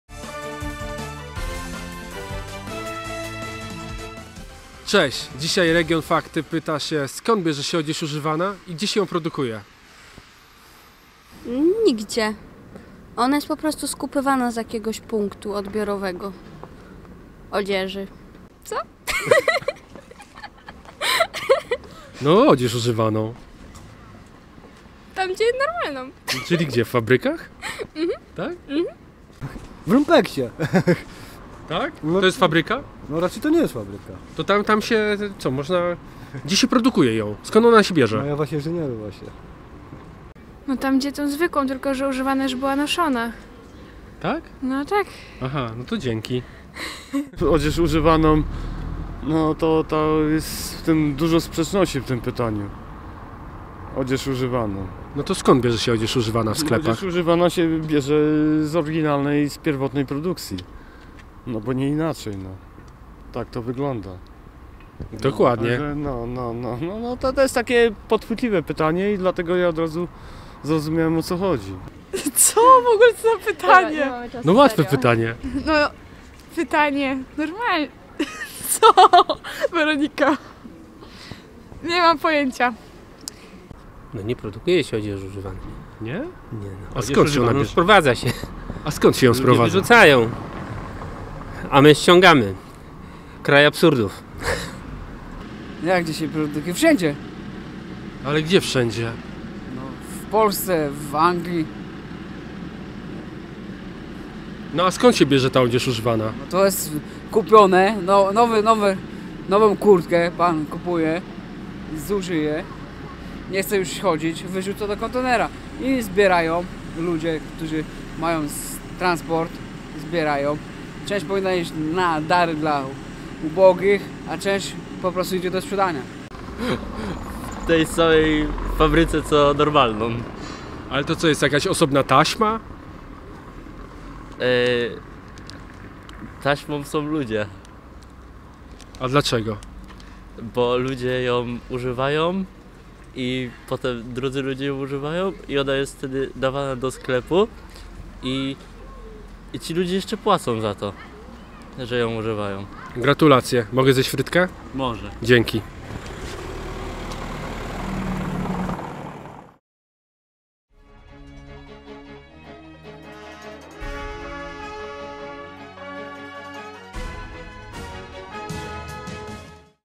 Skąd się bierze odzież używana (SONDA)
Pytamy świdniczan, skąd się bierze odzież używana.